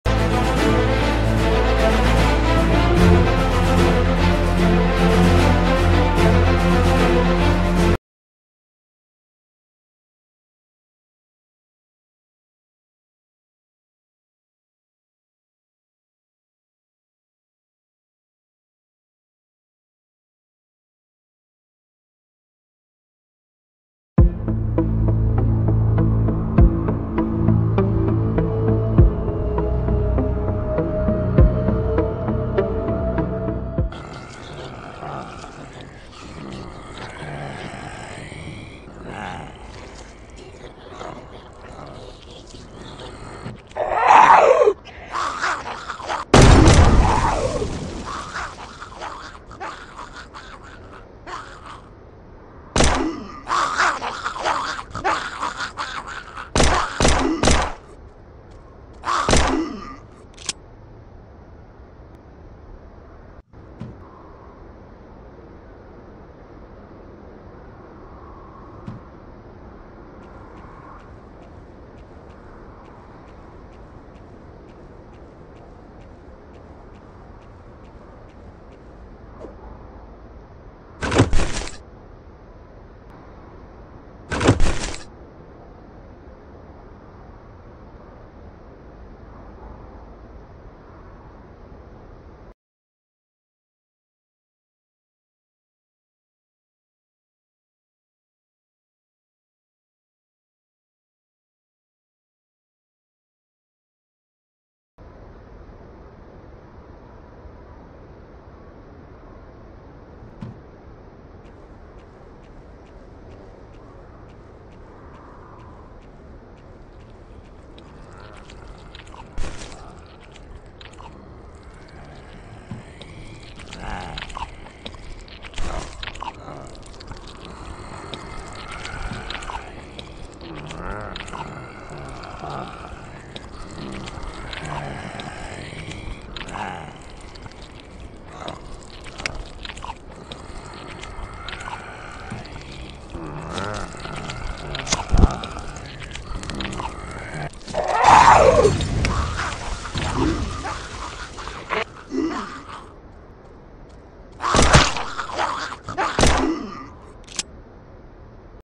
offline zombie games for Android of 2023 | zombie shooting offline android games download
Hi guys .

i have bring a new type of zombie shooting and survival game for Android

best zombie shooting & survival game with high graphics in just 150 mb size